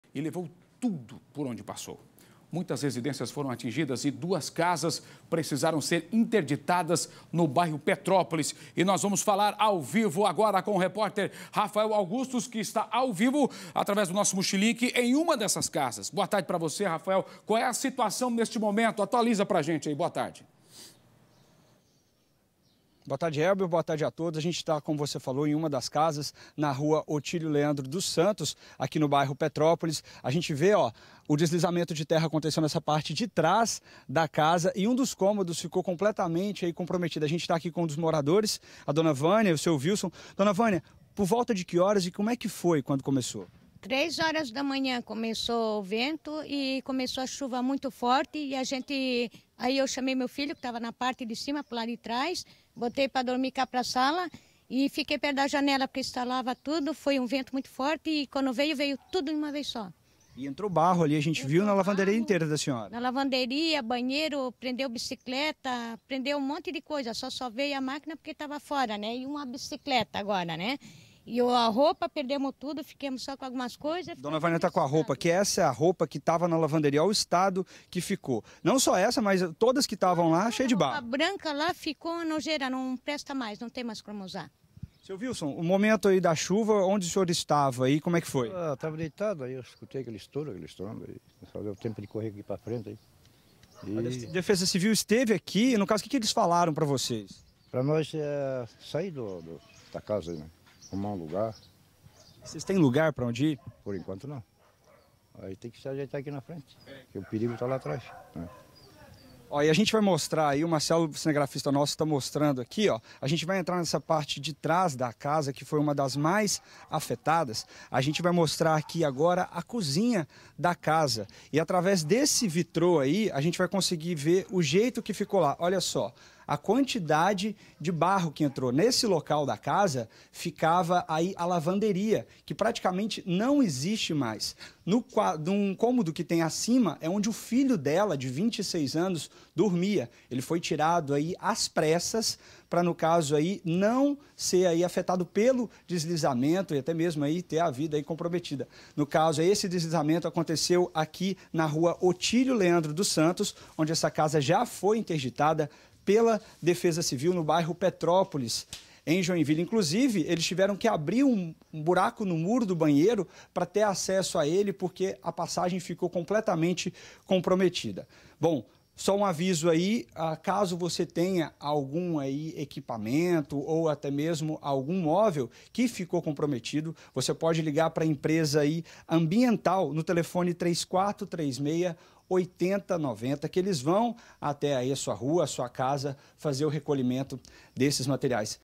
Moradores de Joinville podem solicitar recolhimento de móveis atingidos pela chuva
A chuva em Joinville não deu trégua e levou tudo por onde passou. Muitas residências foram atingidas e duas casas precisaram ser interditadas no bairro Petrópolis. O repórter Raphael Augustus esteve no local.